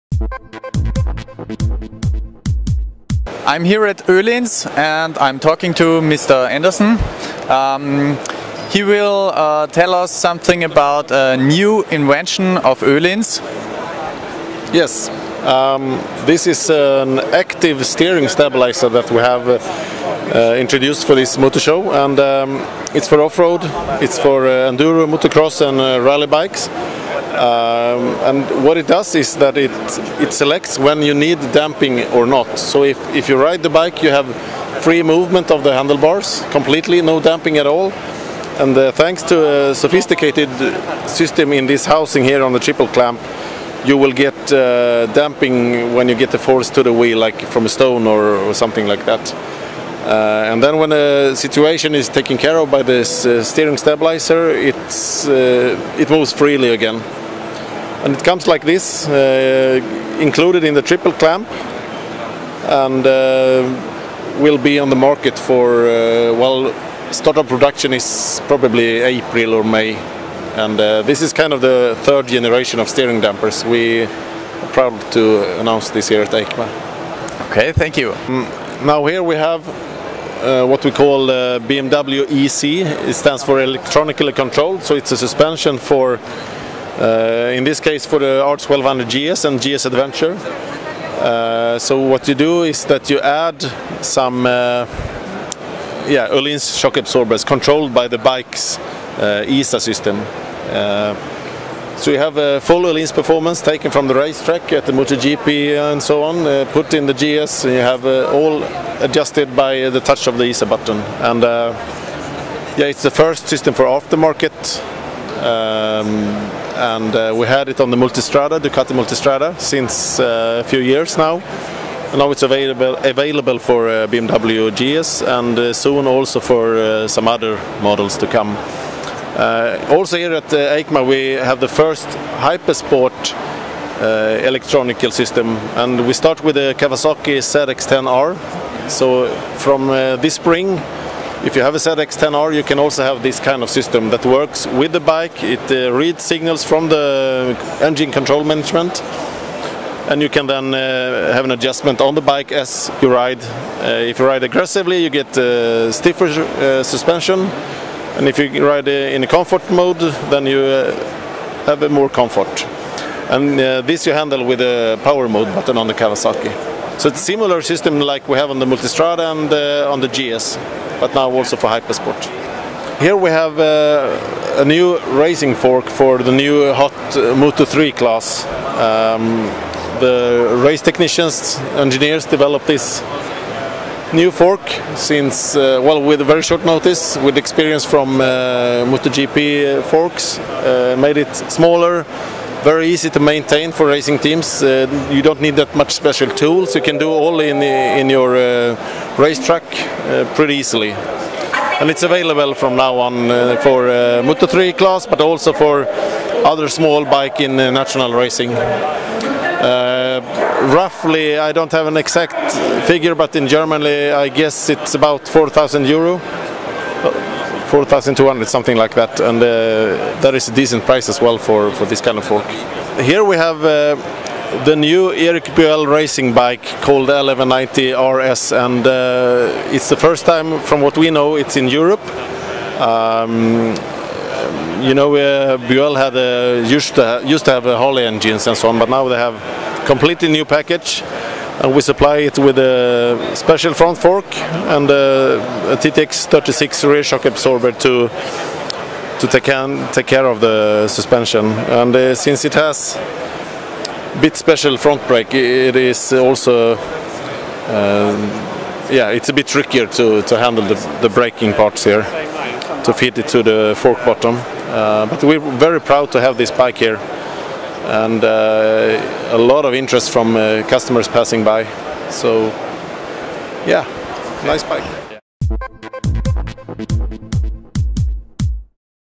Öhlins Suspension New Products 2012
Öhlins stellte auf der EICMA neue Innovationen vor. Insgesamt gab es gleich 11 Neuheiten zu besichtigen:
Road & Track (Straße):

    ÖHLINS Elektronisch kontrolliertes Fahrwerk für die BMW R1200 GS/Adventure
    ÖHLINS manuell einstellbares TTX-Fahrwerk für die BMW R1200 GS/Adventure (erstmalig mit TTX39-Federbein)
    ÖHLINS „Street Performance Line" S46DR1-Stoßdämpfer für weitere Modelle lieferbar
    Nachrüst-Telegabel FGRT 200 NEU

Racing (Straße):

    ÖHLINS Nachrüst-Superbike-Telegabel FGR 200 NEU
    ÖHLINS Nachrüst-Racing-Telegabel FGR 385 für Moto3 NEU
    ÖHLINS TTX36-Mk2-Stoßdämpfer Modell 2012

Off Road:

    ÖHLINS selektiver MX-Lenkungsdämpfer S3 NEU
    ÖHLINS TTX44-Mk2-Stoßdämpfer Modell 2012
    ÖHLINS 48mm-MX-Nachrüst.Telegabel Modell 2012
    ÖHLINS ILX30-Stoßdämpfer für Trial, ATV etc. NEU